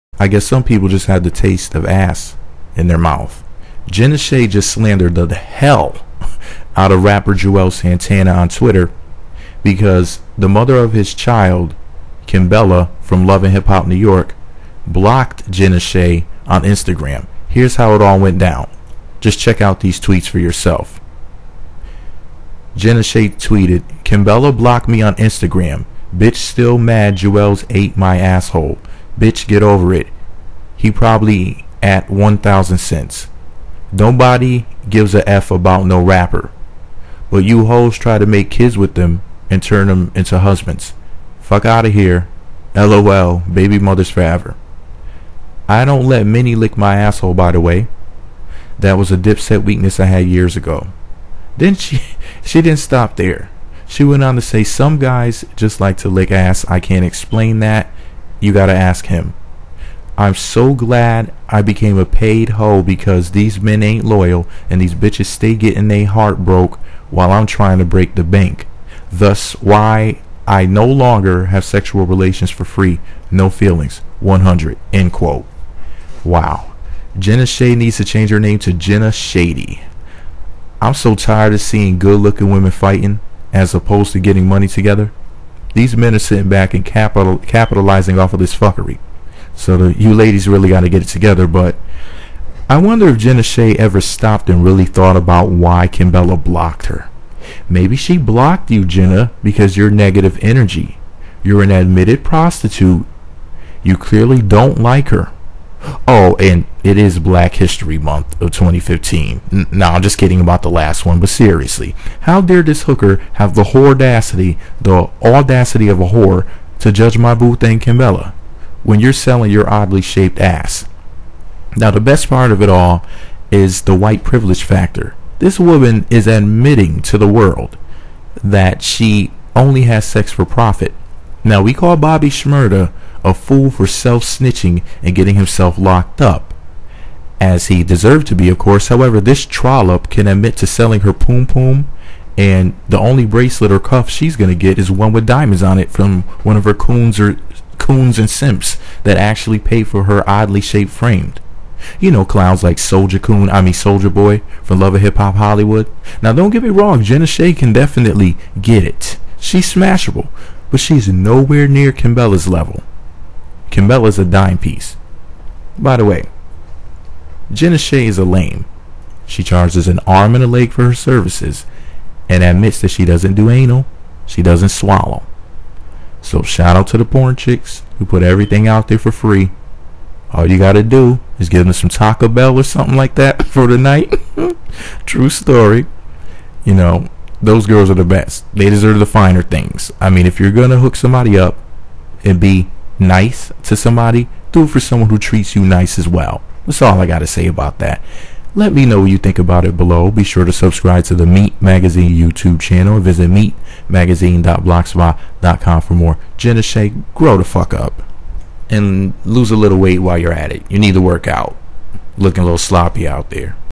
Jenna Shea shades Kimbella | Love and Hip Hop New York | Kim blocked her on Instagram!
Jenna Shea shades Kimbella (Love and Hip Hop New York) | Kim blocked her on Instagram! Hooker claims Juelz ate her asshole! I'm talking Kevin Gates style!

Sometime, they just have a taste for ass!

Jenna Shaw just slandered the hell out of rapper Juelz Santana on Twitter after his on and off again girlfriend and mother to his children, Kimbella Vanderhee, blocked the boxum blonde on Instagram!
Here's how it all went down! Just check out these tweets from
Jenna Shae!

(Screen Captures on blog and in this video!_

Wow! Jenna Shae needs to change her name to Jenna Shady!
I'm so tired of seeing good looking women fighting as opposed to getting money together!
These men are sitting back capitalizing off of this fuckery! Get it together ladies, please!

I wonder if Jenna ever stopped and really thought about why Kimbella blocked her.
Maybe she blocked you Jenna because you're negative energy, you're an admitted prostitute, you clearly don't like her, oh, and because it's black history month! Nah, just kidding about that last one!

But seriously!

How dare this hooker have the Whoredacity, the audacity of a whore to judge my boo Kimbella when you're selling your oddly shaped ass?

Now the best part of it all is the Whit Privilege factor! This woman is admitting to the world that she only has sex for profit. Now, we call Bobby Shmurda a fool for self snitching and getting himself locked up, as he deserved to be of course. However, this trollop can admit to selling her poom poom and the only "bracelet" or cuff she will get is one with diamonds on it from her coon SIMP's that actually pay for oddly shaped frame!

You know, clowns like Soulja Boy from Love and Hip Hop Hollywood!

Don't get me wrong, Jenna Shae is definitely smashable. But she is no where near Kimbella's level! Bae is a dime piece!

PS. Jenna Shae is lame! She charges an arm and a leg, she doesn't do anal, and she doesn't swallow.
Shout out to my porno chicks who put out for free and do everything. All you gotta give some of them is some Taco Bell for the night, true story!

Porn stars are the best! They deserve the finer things!
I mean, if you're gonna do something nice for somebody, do it for someone who treats you nice!

That's all I gotta say about that!

Let me know what you think about it all below!